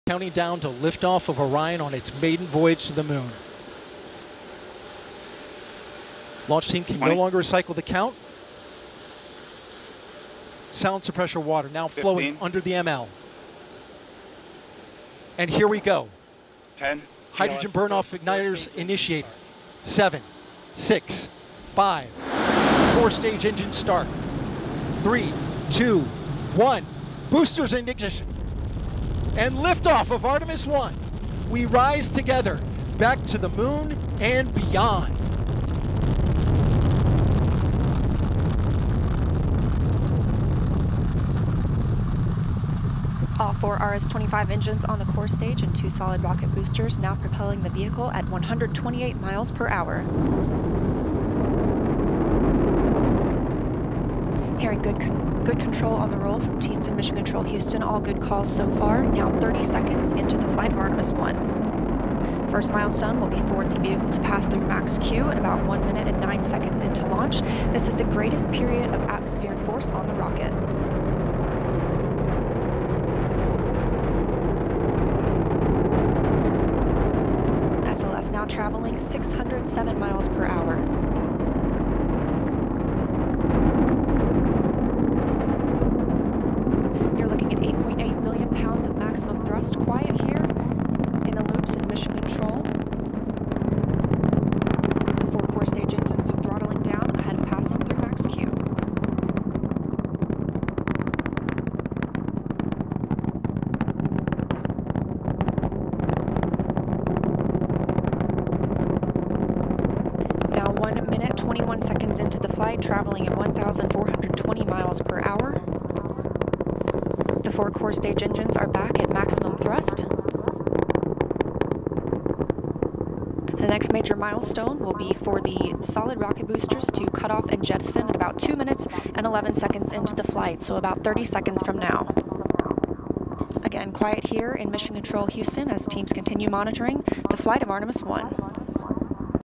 NASA Artemis 1 Launch
"And lift off of Artemis 1. We rise together, back to the moon and beyond."